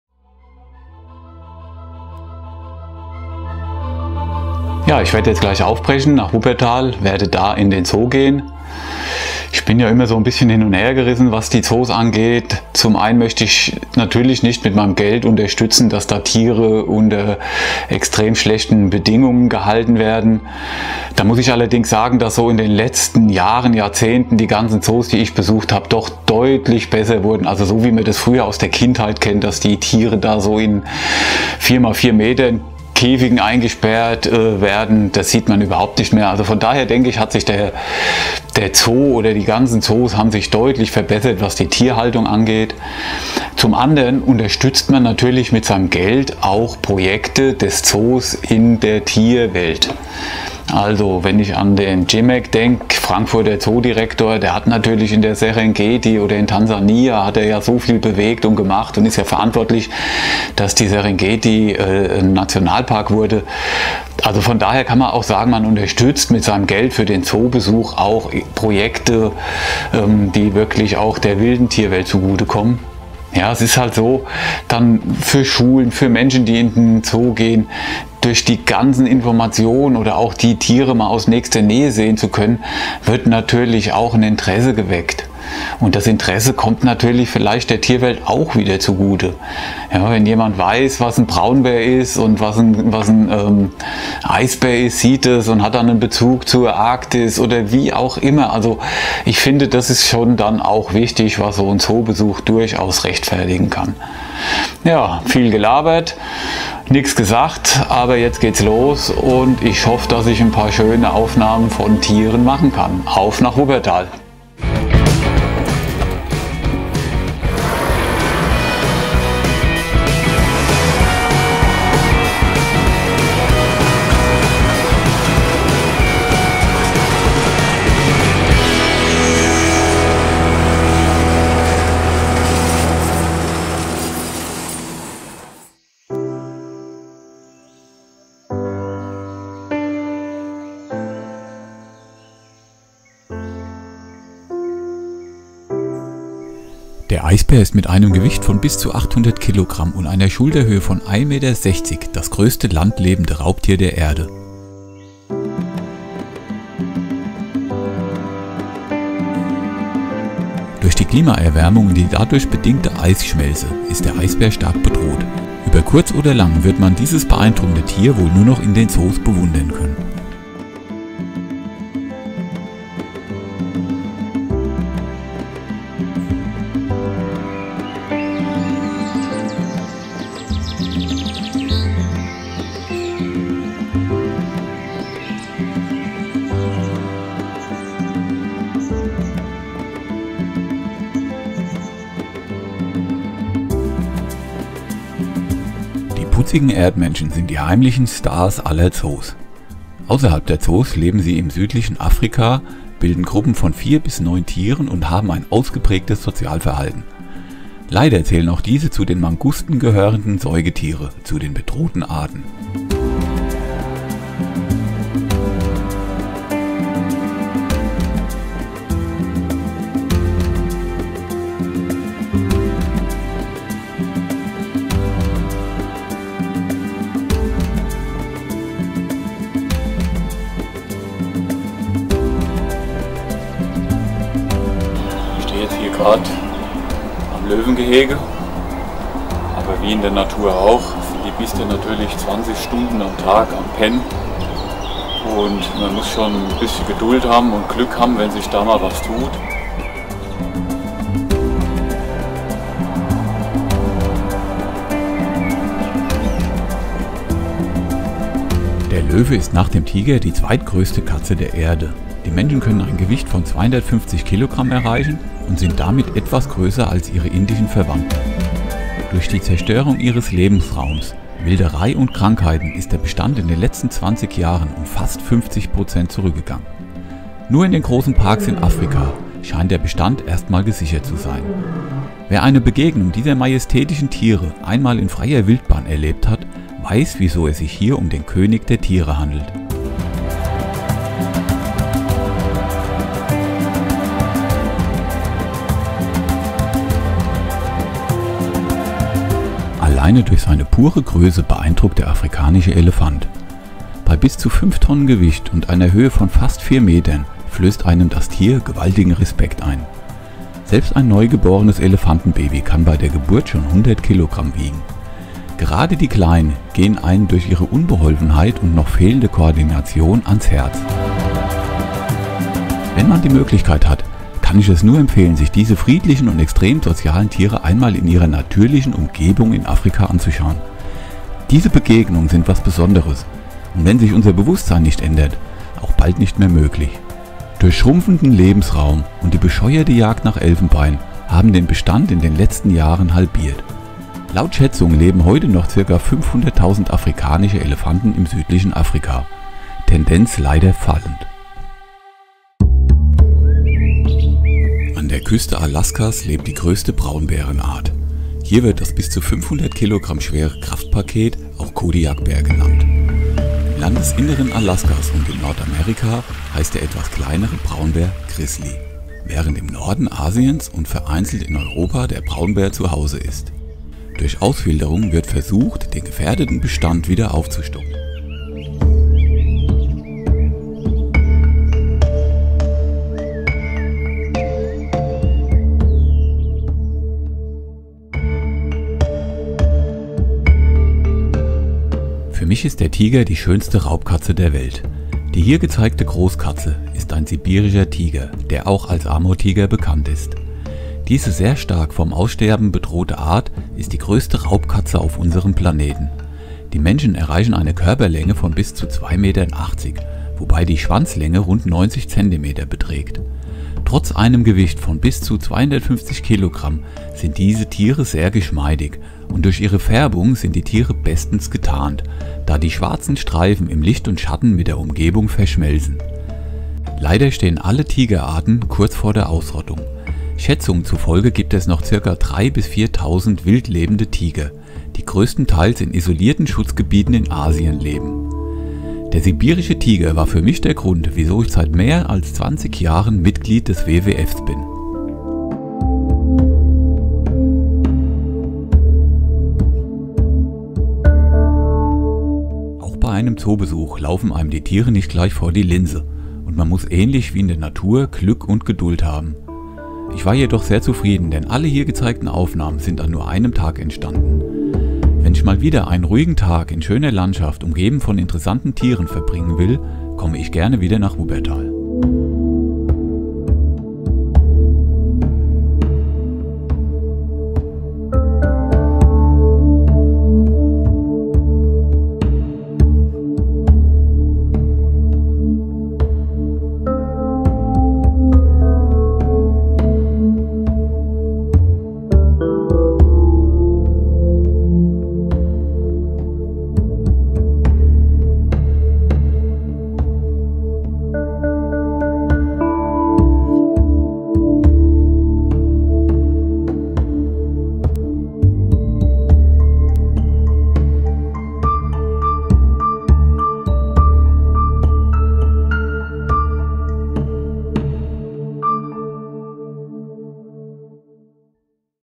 Der grüne Zoo Wuppertal
Einfach mal raus, etwas Natur, Tiere und das Hobby genießen. Was bietet sich das besser an als ein Zoobesuch. Der Wuppertaler Zoo ist dafür wie gemacht und ich hatte Glück und einige Tiere zeigten sich perfekt vor der Kamera.
Viel Spaß